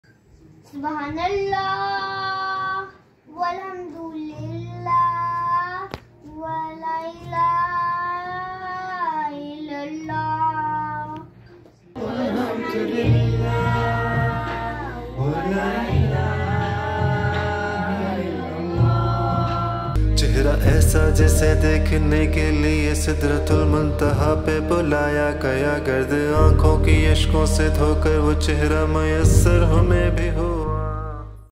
Naat - BTS & Promo
Loved recording this Naat performed by Mr. Tayyab Sheikh and his children, Masha Allah.